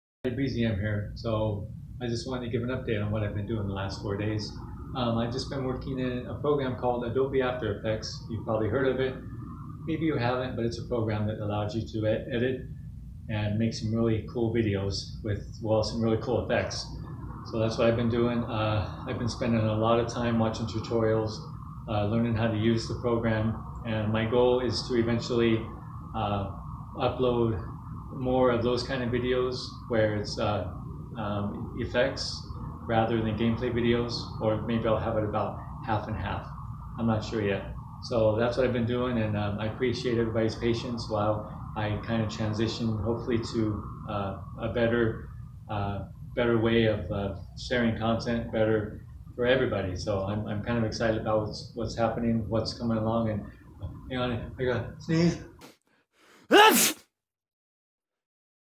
And BreezyM Was No More
Hey everyone. I just wanted to update you all of what I've been doing over the last few days besides making videos. I've learned a lot with Adobe After Effects but there's still a long way to go. Thanks for all of the amazing support on this channel.